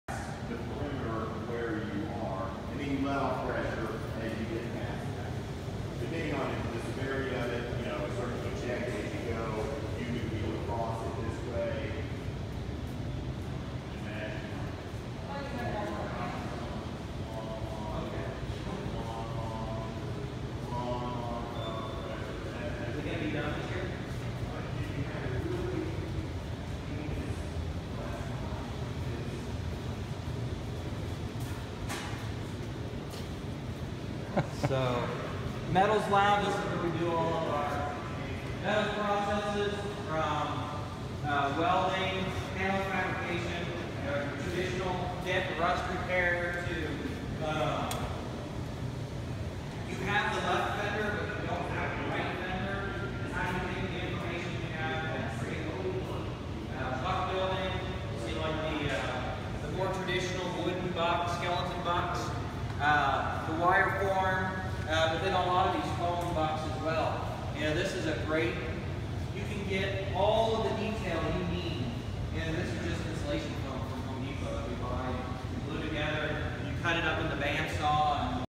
Custom Coachworks jigs bucks English wheels at McPherson College (Kansas) Auto Restoration Clip 8
Custom Coachworks jigs bucks English wheels